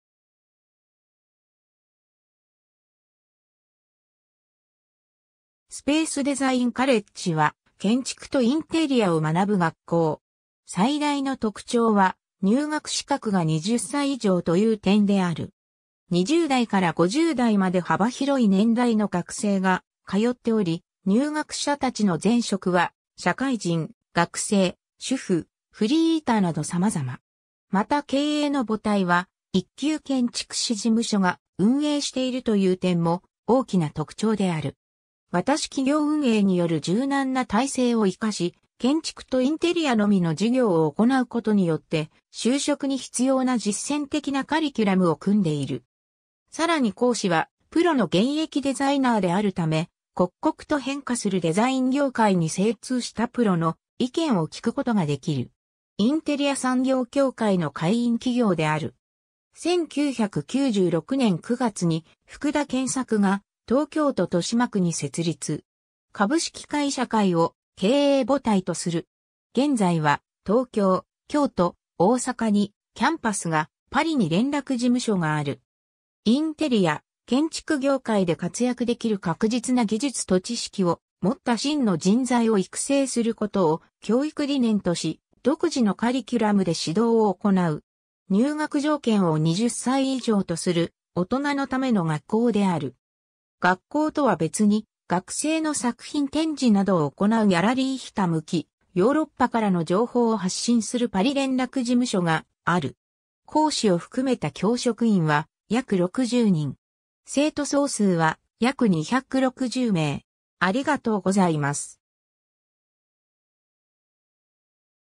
スペースデザインカレッジ
スペースデザインカレッジは、建築とインテリアを学ぶ学校。
 最大の特徴は「入学資格が20歳以上」という点である。
 20代から50代まで幅広い年代の学生が通っており、入学者達の前職は社会人・学生・主婦・フリーターなど様々。
 また経営の母体は、一級建築士事務所が運営しているという点も大きな特徴である。
 私企業運営による柔軟な体制を活かし、建築とインテリアのみの授業を行うことによって、就職に必要な実践的なカリキュラムを組んでいる。
 さらに講師はプロの現役デザイナーであるため、刻々と変化するデザイン業界に精通したプロの意見を聞くことができる。
 インテリア産業協会の会員企業である。
 1996年9月に福田健策が東京都豊島区に設立。
株式会社カイを経営母体とする。
 現在は東京、京都、大阪にキャンパスが、パリに連絡事務所がある。
 インテリア・建築業界で活躍できる確実な技術と知識を持った真の人材を育成することを教育理念とし、独自のカリキュラムで指導を行う。
入学条件を20歳以上とする大人のための学校である。
 学校とは別に、学生の作品展示などを行うギャラリーひたむき、ヨーロッパからの情報を発信するパリ連絡事務所がある。
 講師を含めた教職員は約60人。
生徒総数は約260名。